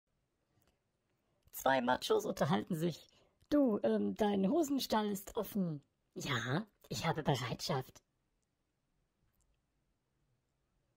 Der machöse Stall
Zwei Machos unterhalten sich: "Du, Dein Hosenstall ist offen!" - "Ja, ich habe Bereitschaft."

Vorgetragen von Witzopus